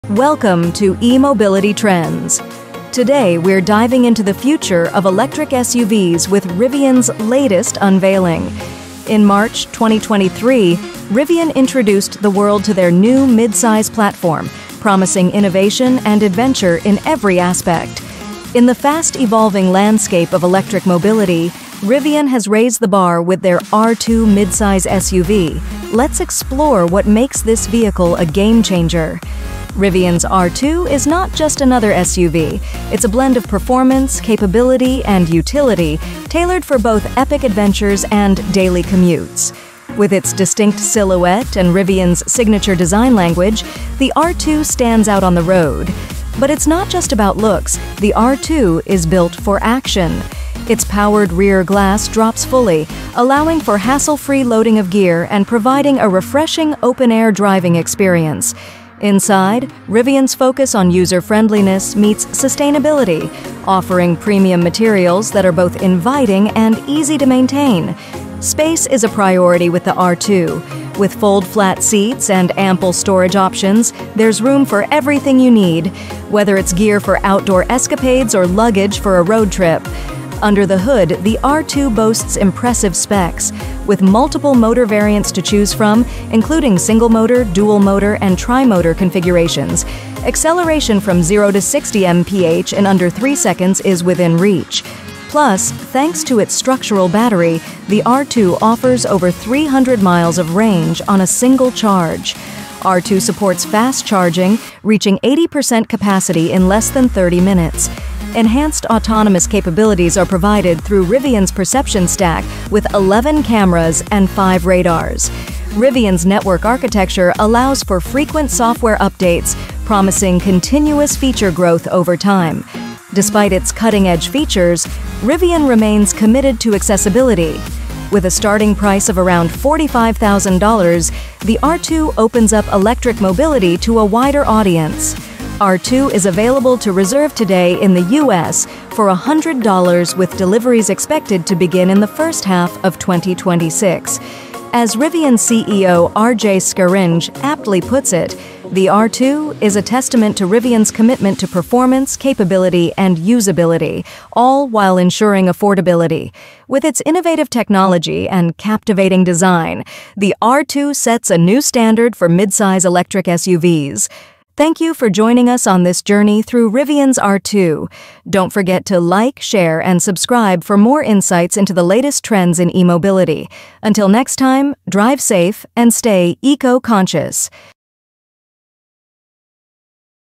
Unveiling Rivian's R2: The Future of Midsize Electric SUV
Join us as we delve into Rivian's groundbreaking R2, a midsize SUV designed for performance, capability, and everyday adventures. Discover its innovative features, including multiple motor variants, a structural battery, and spacious interior, all at an accessible price point.